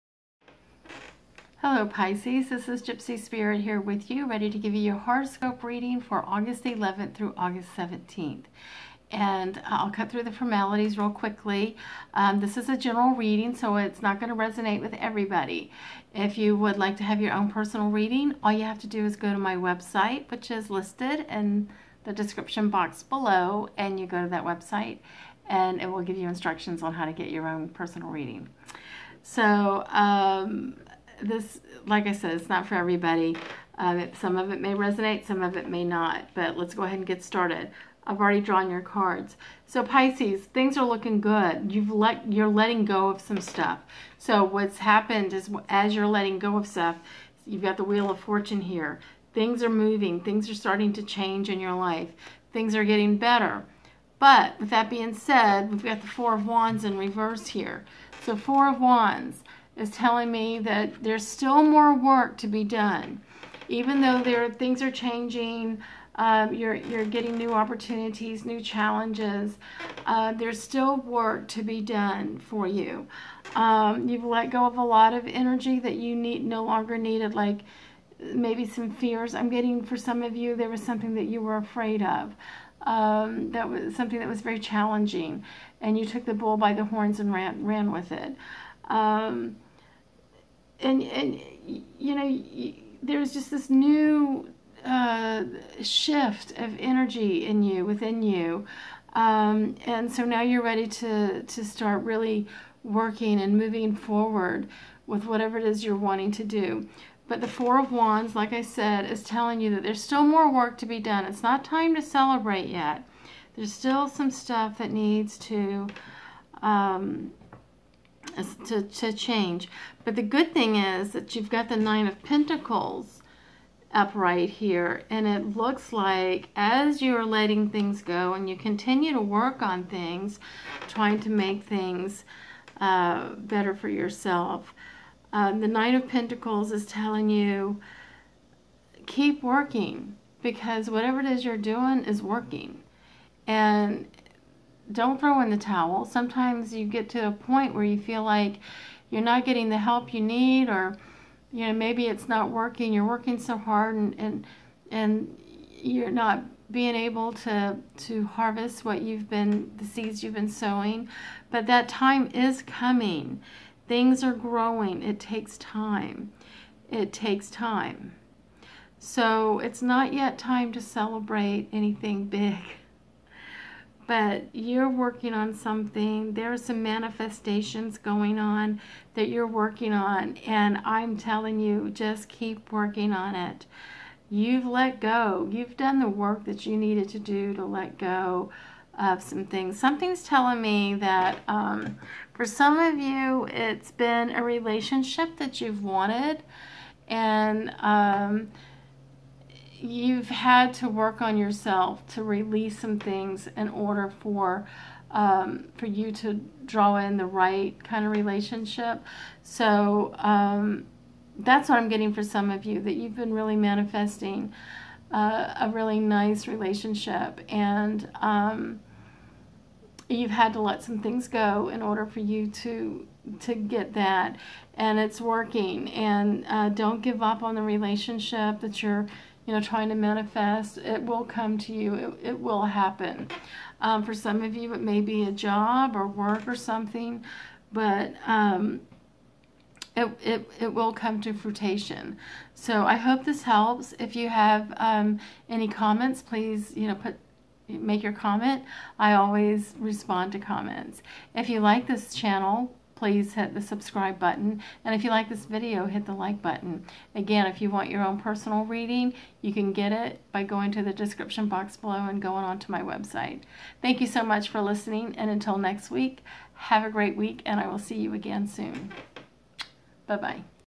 Pisces Horoscope 8/11-8/17, Keep Working, Things Are Looking Good
Gypsy Spirit reads tarot for Pisces for the week of 8/11-8-17. Get your own personal reading, visit